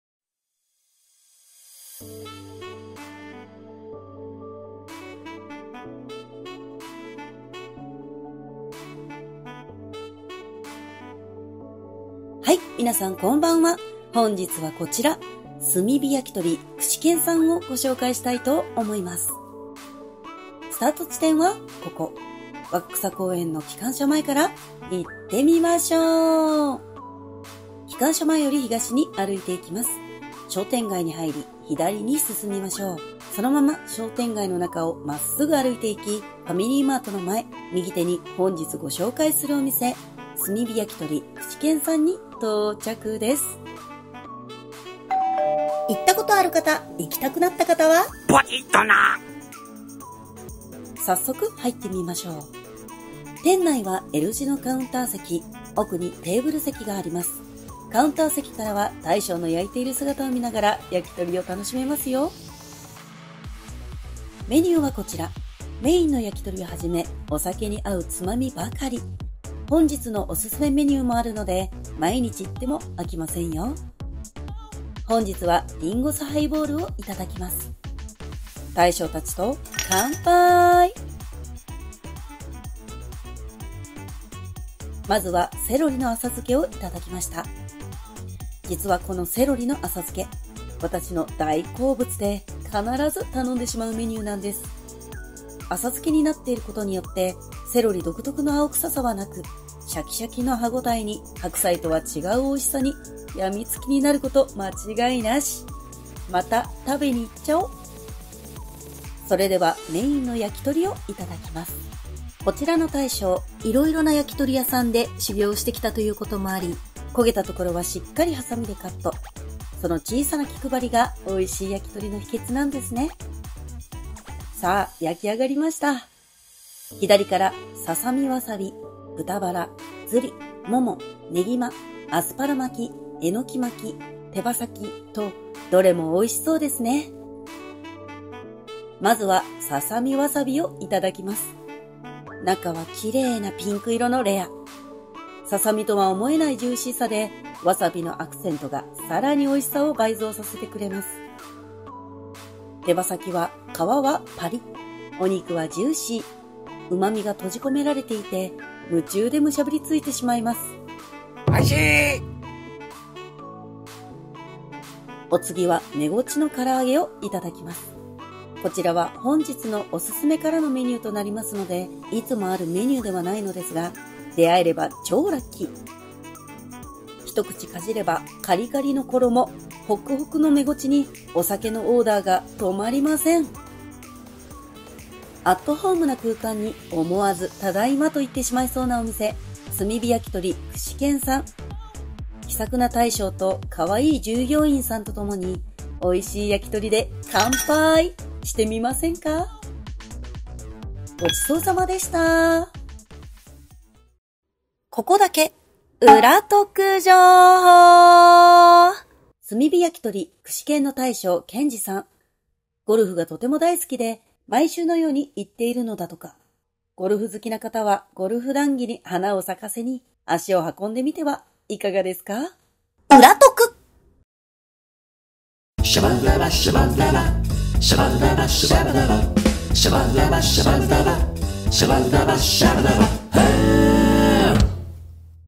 ささみの常識を超える焼き鳥！しっとりジューシーで美味過ぎた 〜炭火焼鳥 串けん〜
大分市中央町にあるお店を紹介していきます！

今回ご紹介するお店は…

【炭火焼鳥  串けん】さん！

焼鳥はもちろん他の一品も美味しすぎてお酒が止まらない⁈
可愛い看板娘もいるので盛り上がる事間違いなしっ！

〒870-0035
大分県大分市中央町1丁目4-13

=前回のお店紹介=